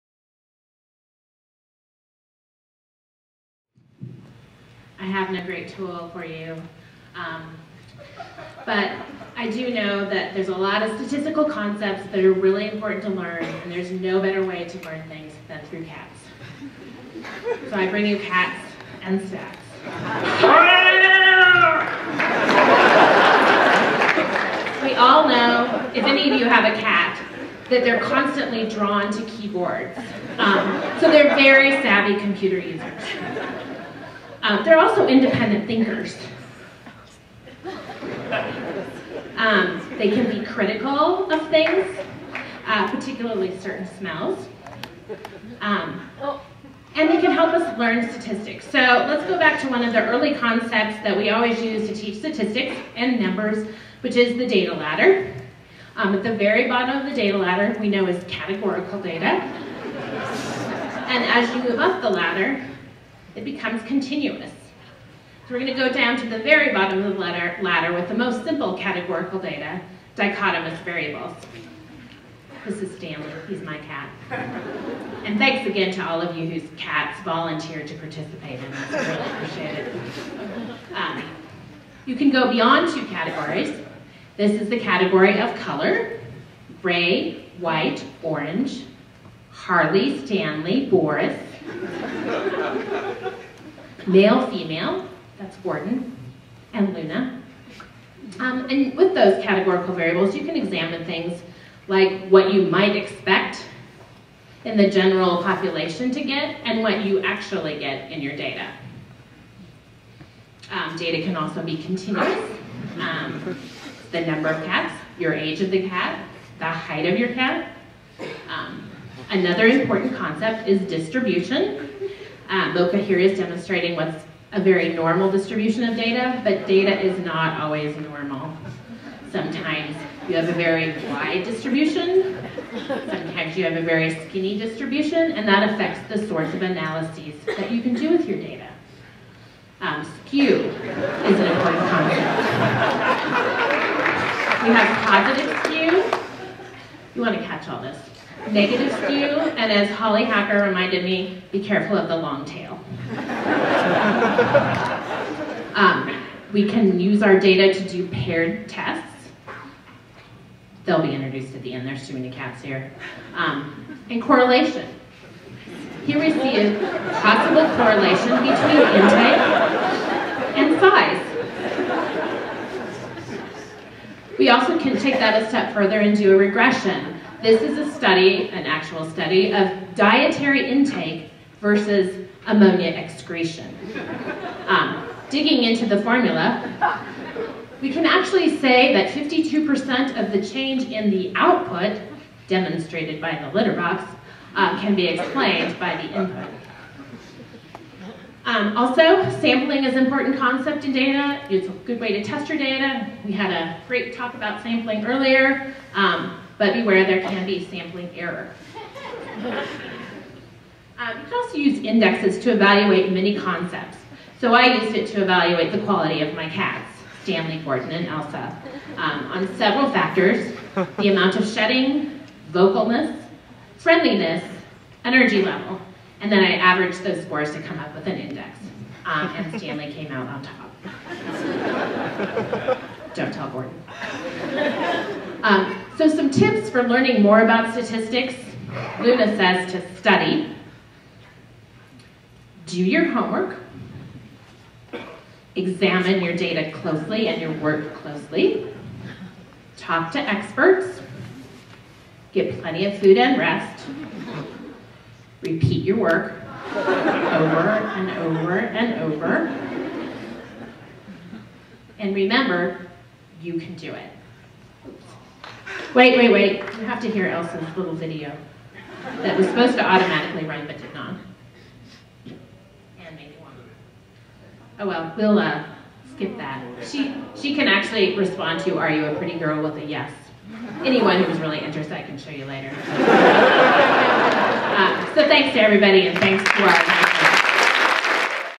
Cats and Stats
Cats and Stats by Jennifer LaFleur

Fresh off the European tour, it's cats AND stats. Stanley will demonstrate dichotomous variables. You'll learn about sampling from Gordon. Harley will give you tips for examining your data. It's not just cats. It's cats AND stats. Learn through the hilarious antics of the cats of NICARians.